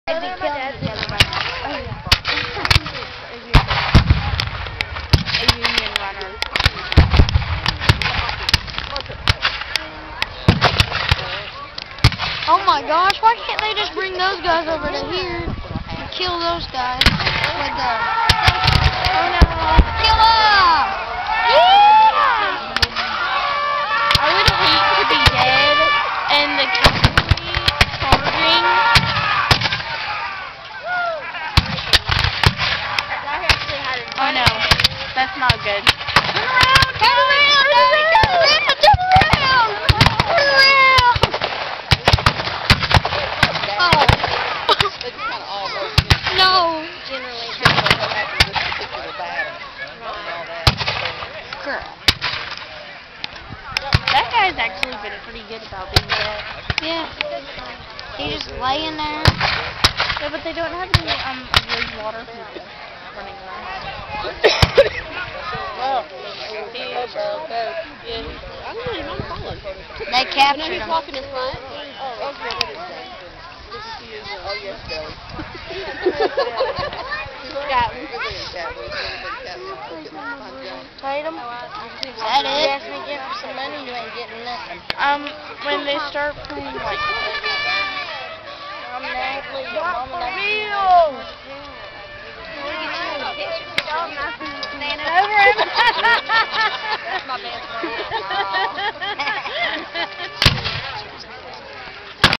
11-3-07 tactical 4
Battle at Nash Farm.  Sorry for the background commentary by our young friend.